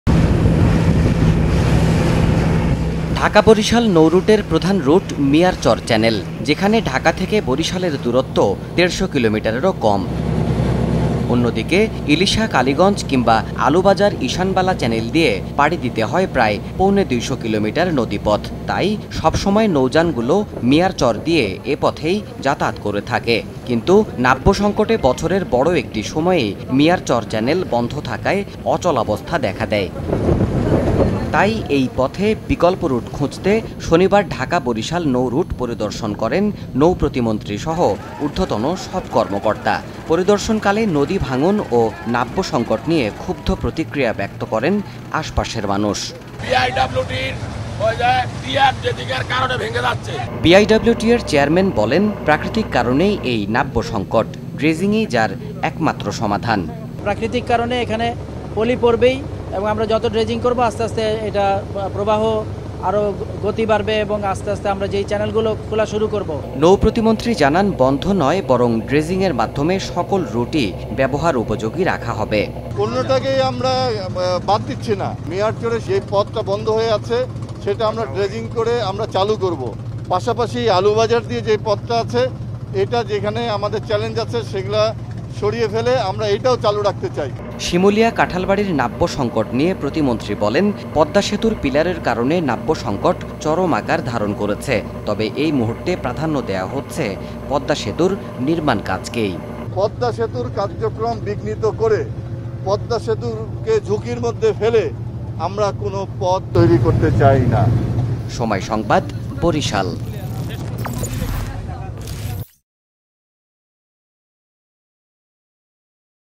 ঢাকা-বরিশাল নৌরুটে চলাচল স্বাভাবিক রাখতে খোঁজা হচ্ছে বিকল্প চ্যানেল | Somoy TV
নাব্য সংকটে ঢাকা-বরিশাল নৌরুটও। চলাচল স্বাভাবিক রাখতে খোঁজা হচ্ছে বিকল্প চ্যানেল। ড্রেজিংয়ের মাধ্যমে সকল রুটই ব্যবহার উপযোগী রাখার চেষ্টা করা হচ্ছে বলে জানিয়েছেন নৌ প্রতিমন্ত্রী। শিমুলিয়া-কাঁঠালবাড়ির নাব্য সংকট নিয়ে তিনি বলেন, পদ্মা সেতুর পিলারের কারণে নাব্য সংকট চরম আকার ধারণ করেছে। তবে এই মুহূর্তে প্রাধান্য দেয়া হচ্ছে পদ্মা সেতুর নির্মাণ কাজকে।

The Dhaka-Barisal route is also in naval crisis. Alternative channels are being sought to keep the movement normal. The state minister said that efforts are being made to keep all the routes usable through dredging. Regarding the naval crisis at Shimulia-Kanthalbari, he said the naval crisis has reached its peak due to the pillars of the Padma Bridge. However, the construction work of the Padma Bridge is being given priority at the moment.